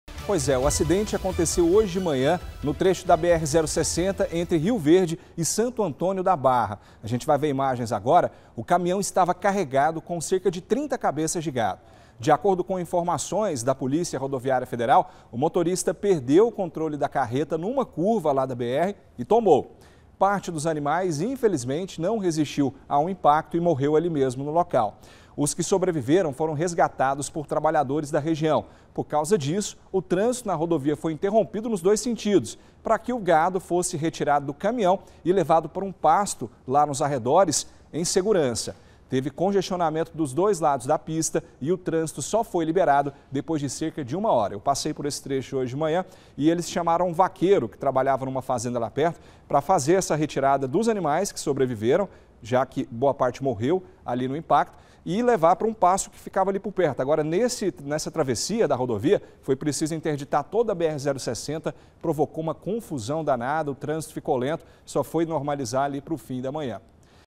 CARREGADO COM GADO: CAMINHÃO TOMBA NA BR-060
UMA CARRETA CARREGADA COM GADO TOMBOU NA "BR-ZERO SESSENTA", NO MUNICÍPIO DE RIO VERDE.